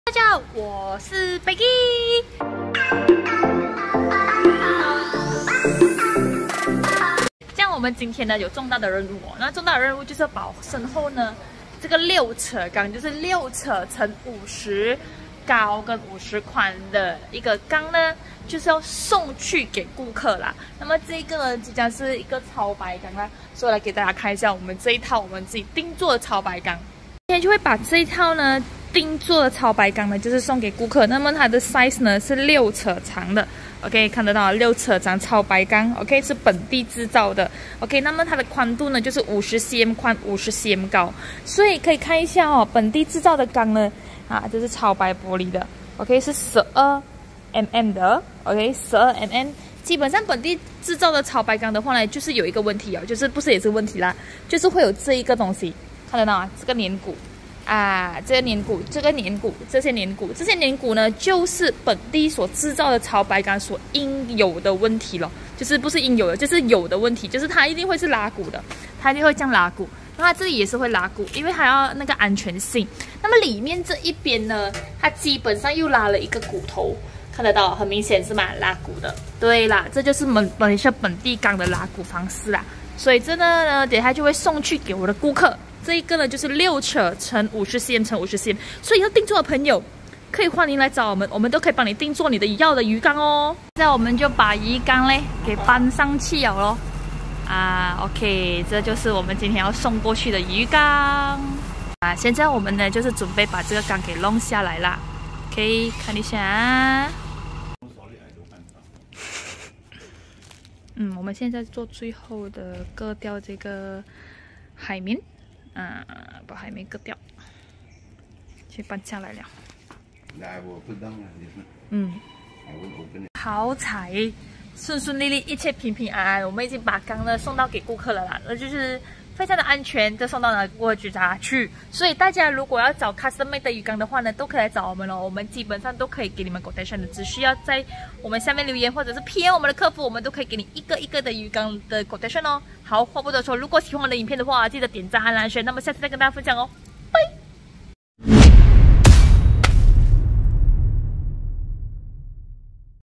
顾客定做超白鱼缸~6尺超白缸运送过程~ 要定做可以找我们
订做超白鱼缸~ 要几尺都可以哦！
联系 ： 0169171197